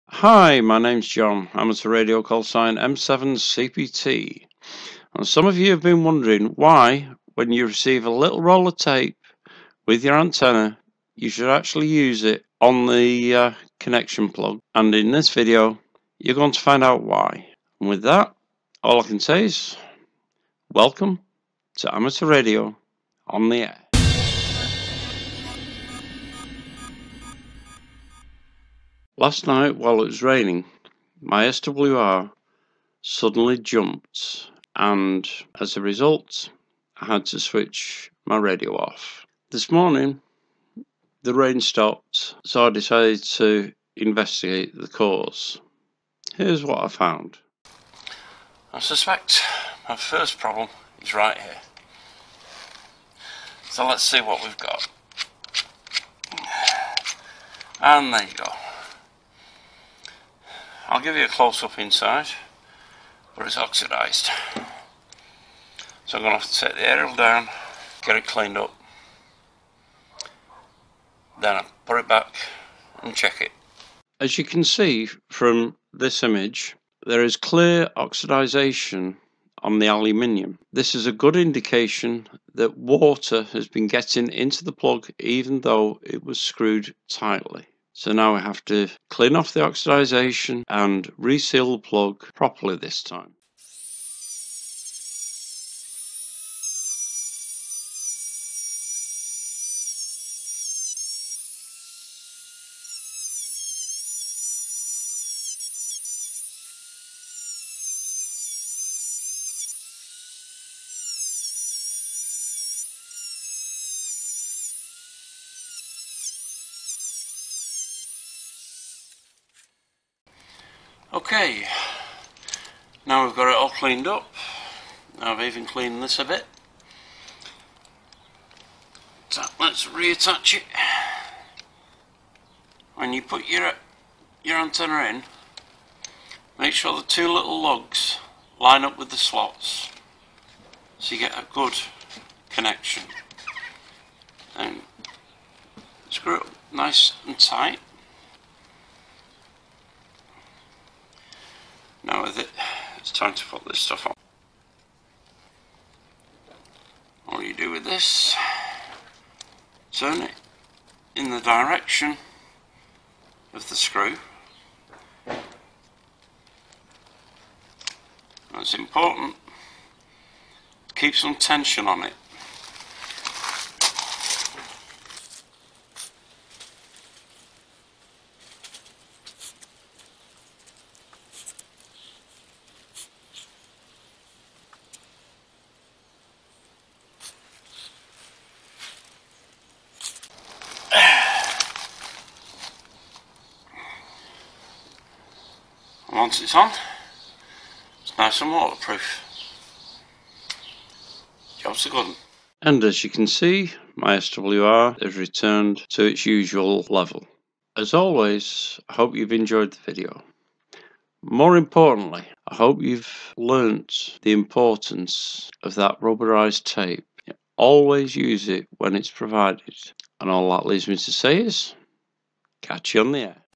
Fixing a sudden SWR change caused by rain
Black Self-Amalgamating Tape, 19mm x 5m, 2 Rolls
Watson WCN-200 - Cross Needle VSWR Power Meter for HF
Watson WCN-400 - Cross Needle VSWR Power Meter for VHF & UHF